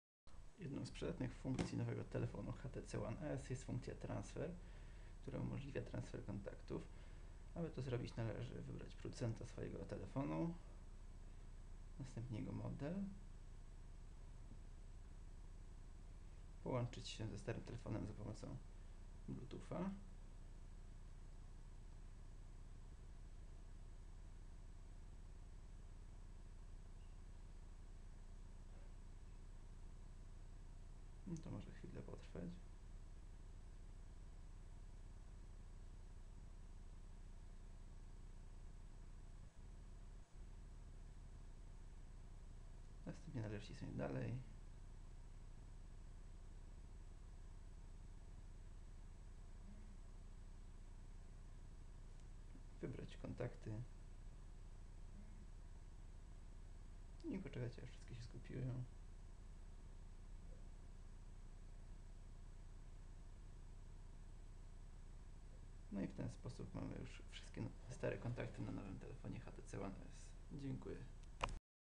Transfer Kontaktów
Przedstawiona funkcja transfer kontaktów na przykładzie HTC One S